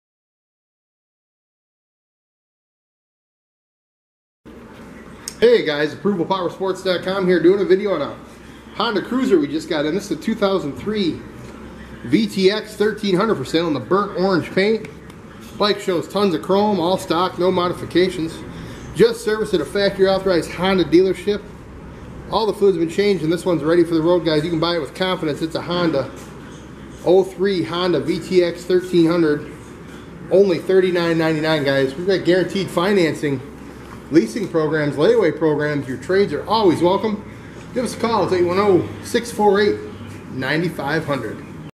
2003 HONDA VTX 1300 U5413
USED HONDA VTX 1300 FOR SALE WITH BURNT ORANGE PAINT AND TONS OF CHROME! ALL STOCK, NO MODIFICATIONS, JUST SERVICED AT A FACTORY AUHTORIZED HONDA DEALERSHIP, ALL FLUIDS CHANGED, INSPECTED AND READY FOR THE ROAD! BUY WITH CONFIDENCE, IT'S A HONDA!